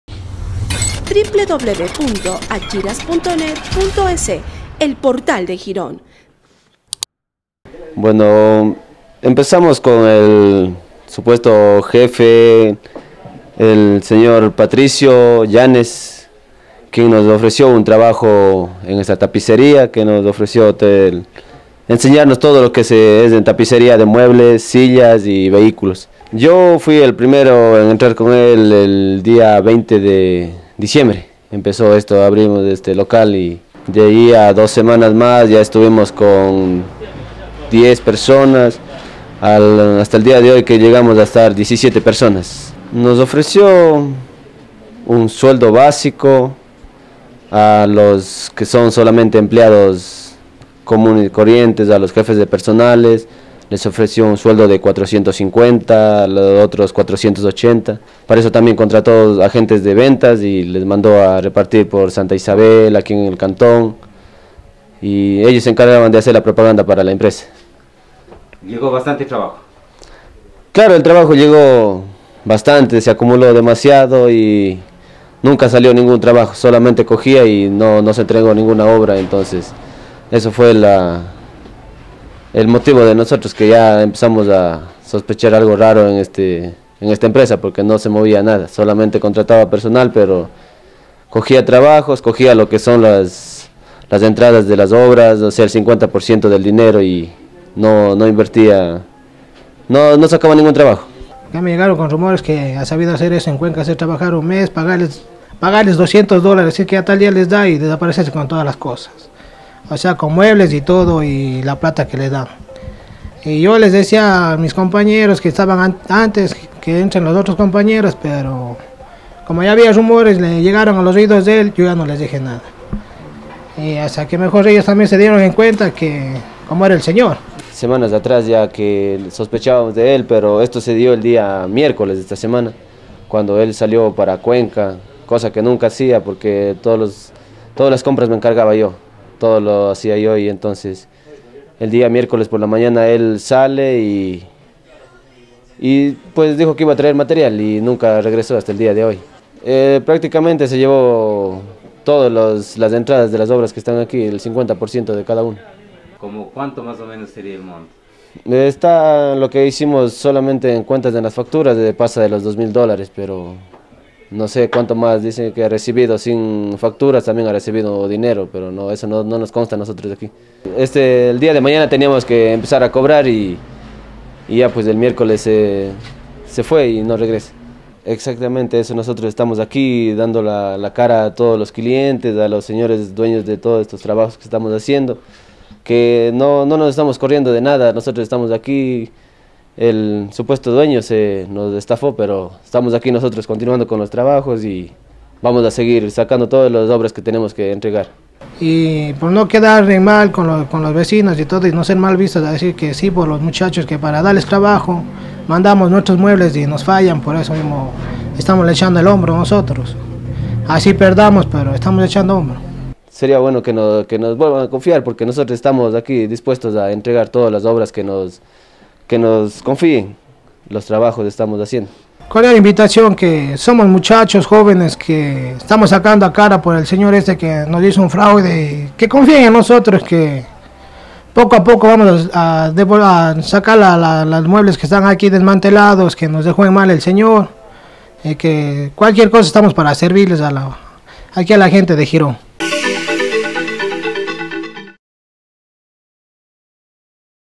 Trabajadores gironenses se sienten engañados por falso empresario tapicero
Son 17 jóvenes de este centro cantonal que se sienten engañados por un supuesto empresario tapicero, que vino a Girón para abrir un negocio de arreglo de muebles pero, luego de coger muchas obras y anticipos desapareció con el dinero, ellos se quedaron con los pendientes, pero dicen, van a dar la cara, afrontar y cumplir con los clientes, y aseguran que tomarán esta situación como una oportunidad.